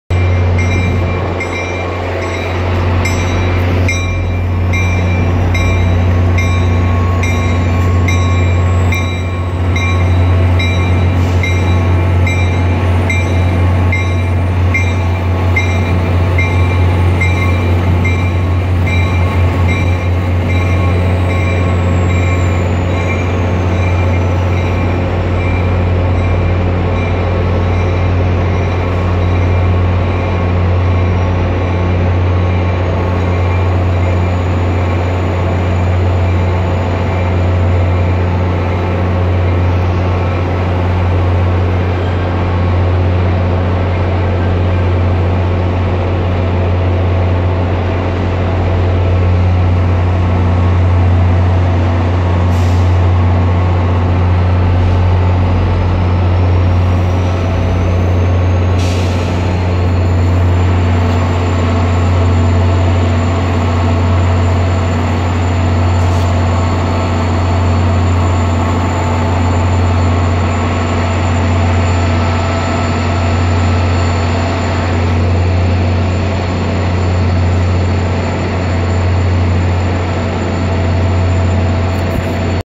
"God dangit, it's that one."
This was recorded at Boston South Station on 4/2/2018, or April 2nd, 2018. The echo coming from the station made me mishear the bell and I pointed the camera at the wrong train... Whoops. But at least it was an F40PH-3C!